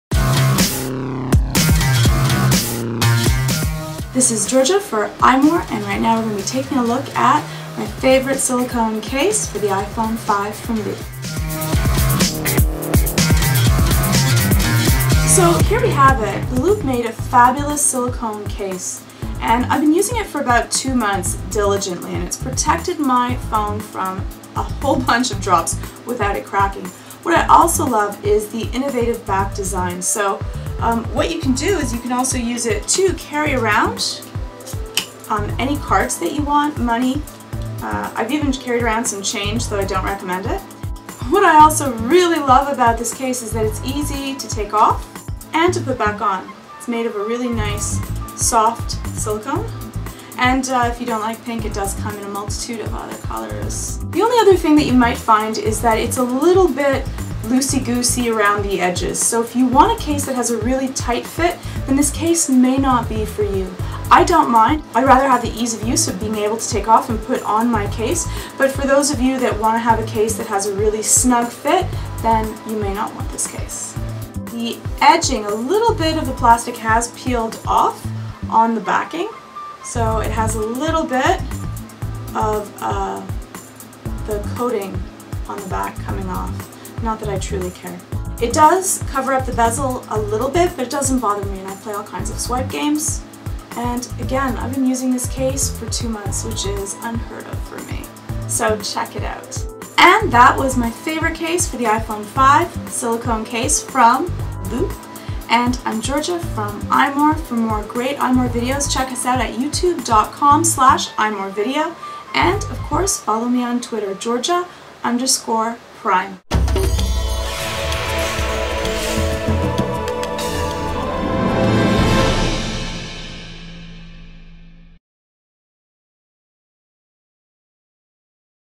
Loop Attachment Mummy case for iPhone 5
I've been using the Loop Attachment Mummy case for iPhone 5 for a couple of months now and while it isn't as shiny or bejeweled as some of my previous case picks, it's amazingly protective and comes in very handy.